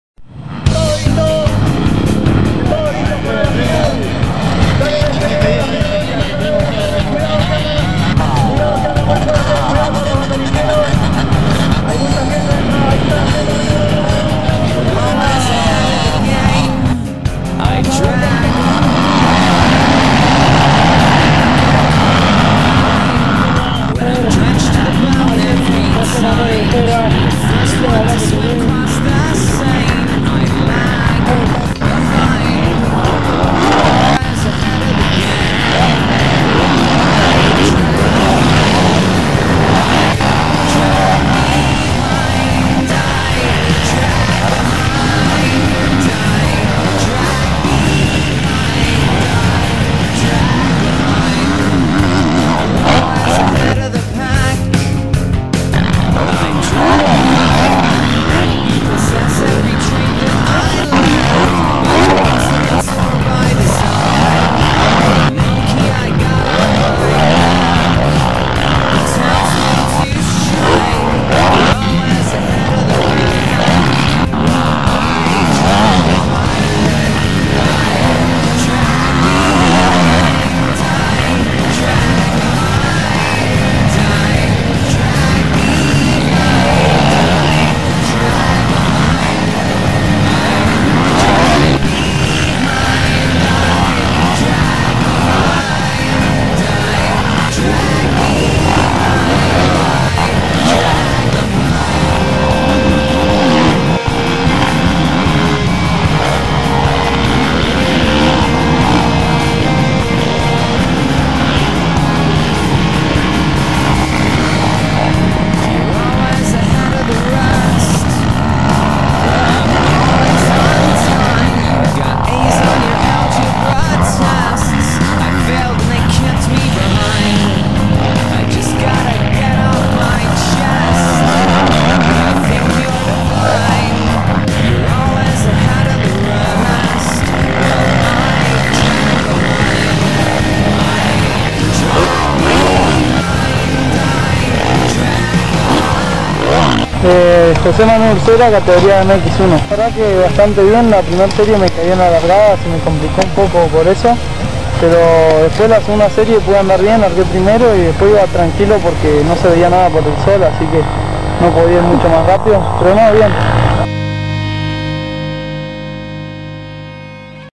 Motocross Fortin Lagunitas
Quinta fecha del certamen del Sur de la República de motocross, que se realizoen el circuito "Fortín Lagunita", de Huergo. El ruido empezo a las 11 con los ensayos, mientras que después se disputarán dos mangas en cada categoría. Las posiciones del certamen: En MX1: 1º) Ezequiel Fanello, 108; 2º) Montero, 74, y 3º) Urcera, 67. En MX2: 1º) Shariff Dipp, 104 puntos; 2º) Matías Sendón, 95, y 3º) Lucas Catoni, 61. En 85 cc: 1º) Nicolás Fanello, 117 puntos; 2º) Joaquín Díaz, 85, y 3º) Giulián Martínez y Víctor Garrido, 77. En másters 1: 1º) Luis Massa, 97 puntos; 2º) Mauro Colafranceschi, 77, y 3º) José L. González, 74. En másters B: 1º) Daniel Bustelo y Daniel Rubio, 84 puntos, y 3º) Marcelo Castillo, 81. En promocionales: 1º) Dante Reggiani, 98; 2º) Nelson López, 71, y 3º) Giuliano Albertón, 69. En enduristas: 1º) Alejandro Molinaro, 79 puntos; 2º) Mariano Muñoz, 61, y 3º) Manuel Loncón, 56.